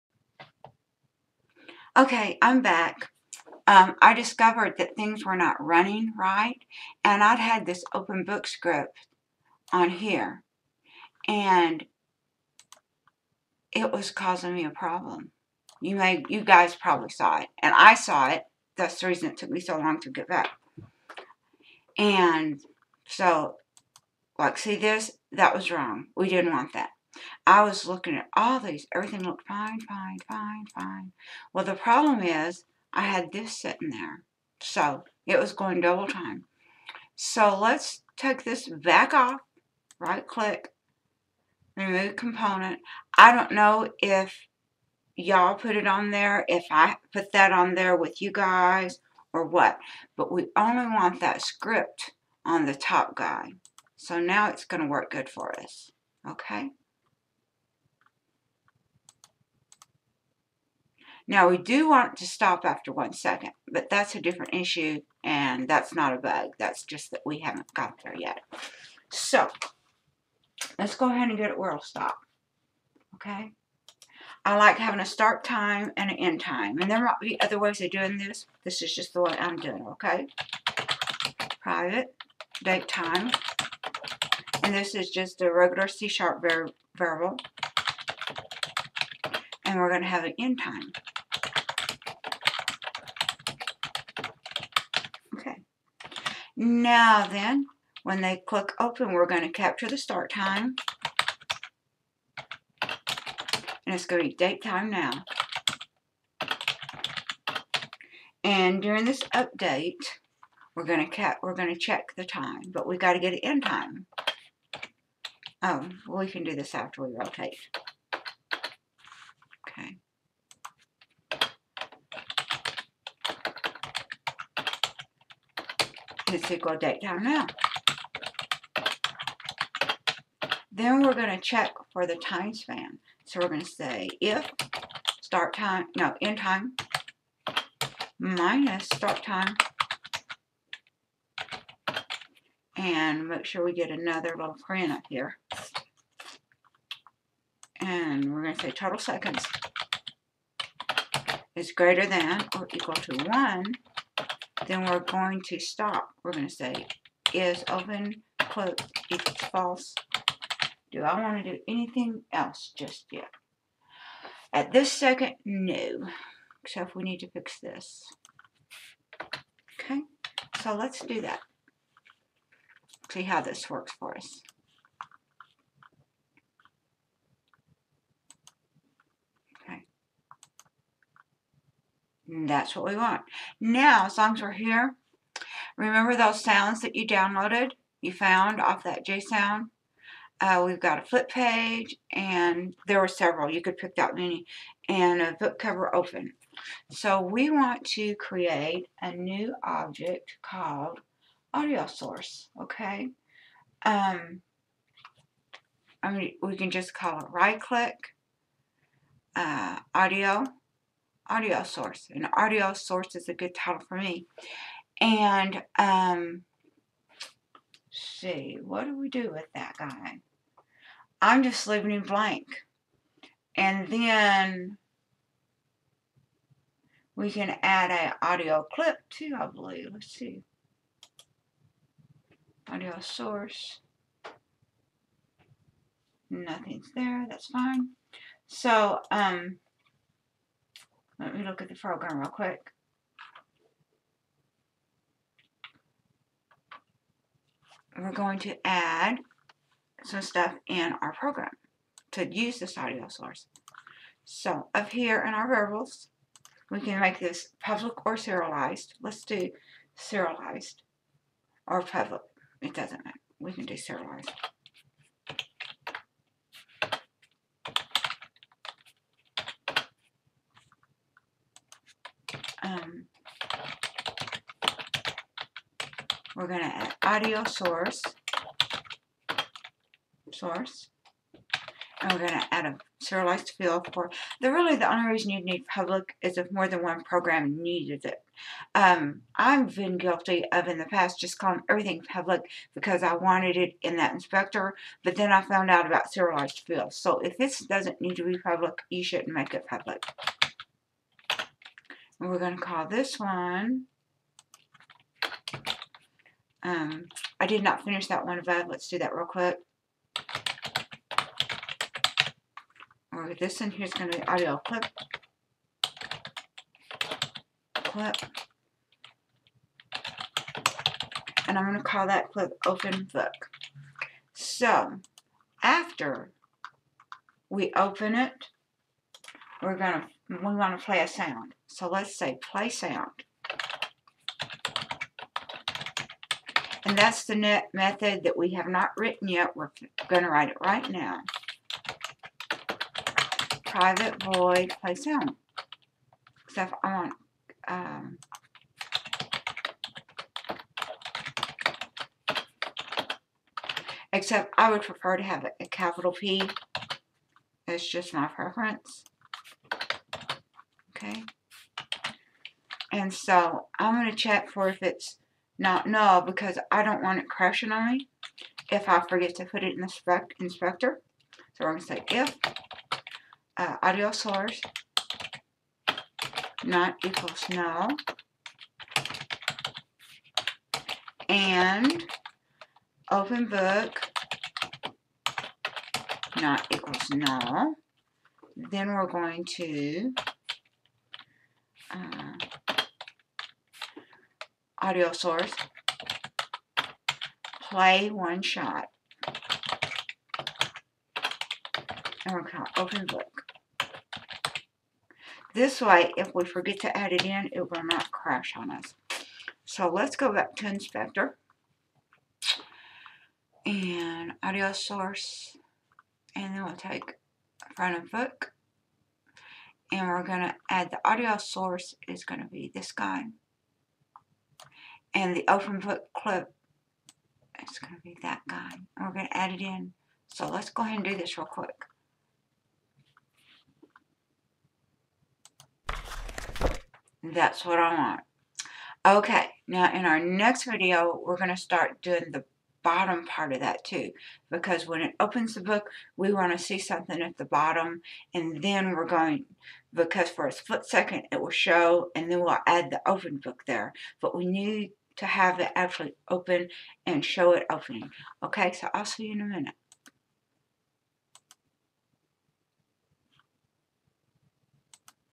C# From Beginner To Pro - Create A FlipBook In Unity3D #7
Open The Front Cover Of The Closed Book And Add Audio

Here is the Code: